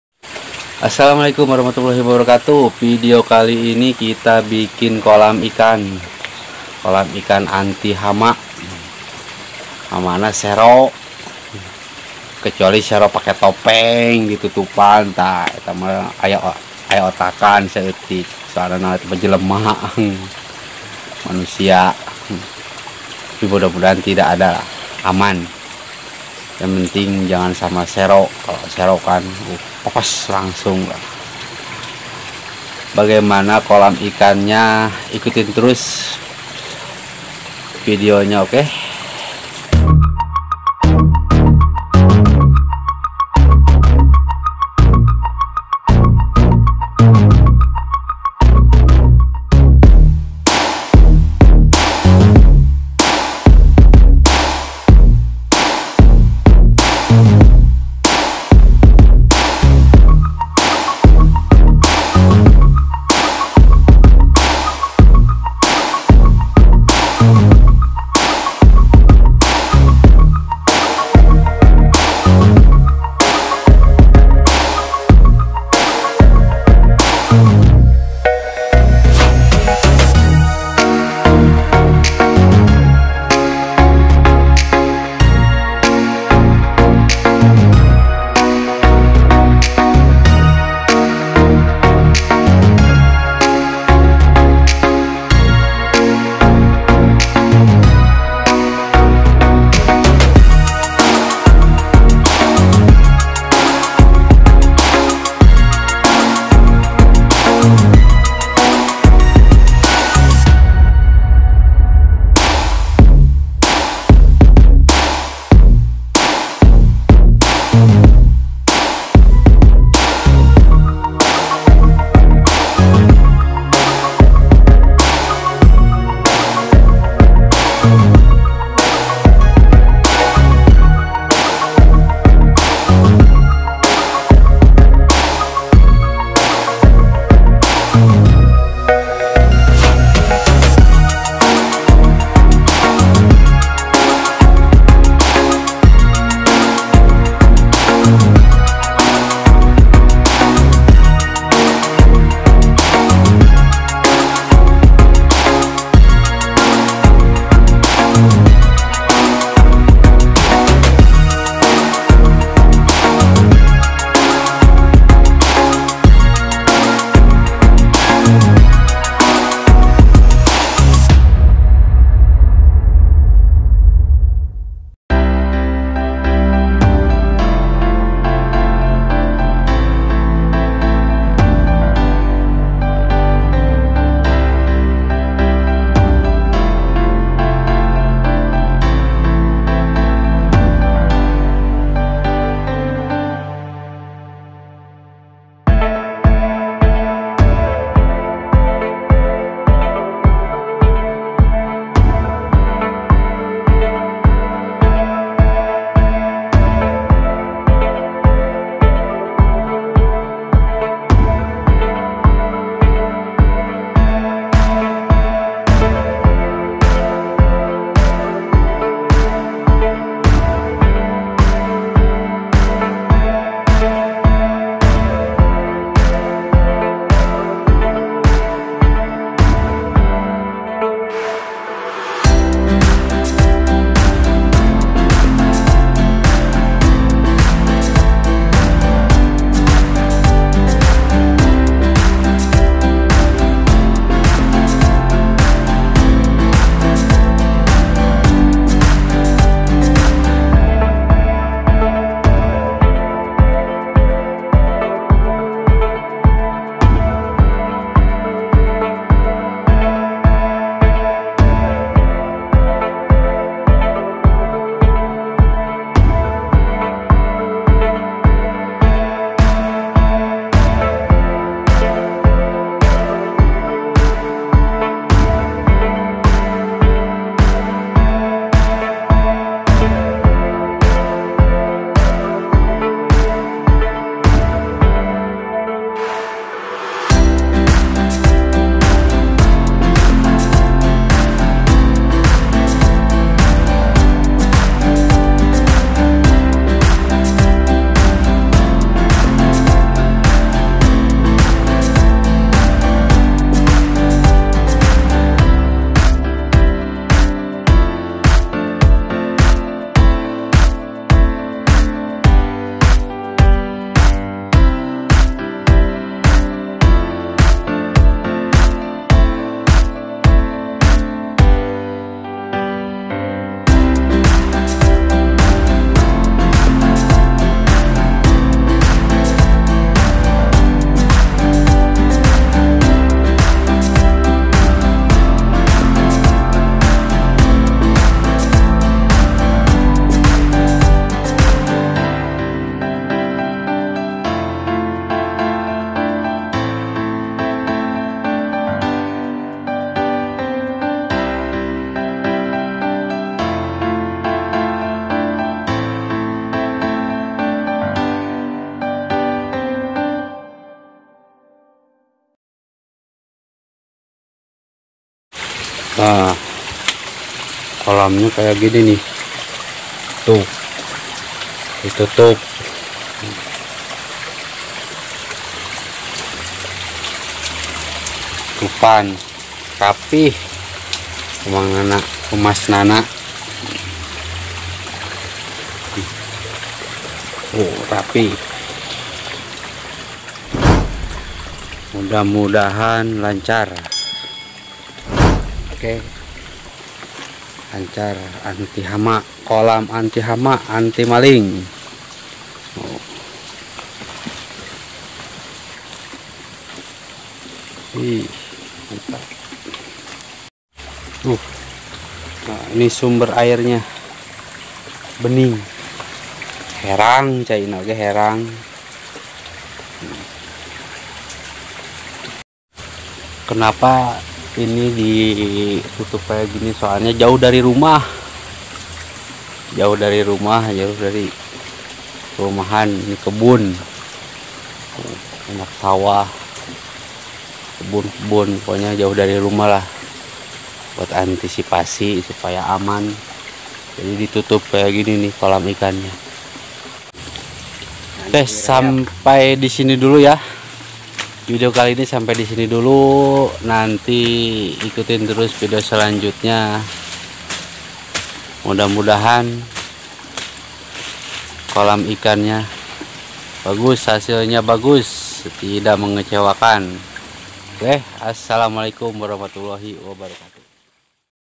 KONSEP KOLAM IKAN ANTI HAMA ANTI MALING
Konsep kolam ikan anti maling ini di buat berdasarkan karna kolam jauh dari rumah atau pemukiman warga,buat anti sipasi aja supaya aman, Bahan nya Bambu sama waring, bambu di buat kaya pagar.. mudah-mudahan manfaat dan menghibur kalian semuanya.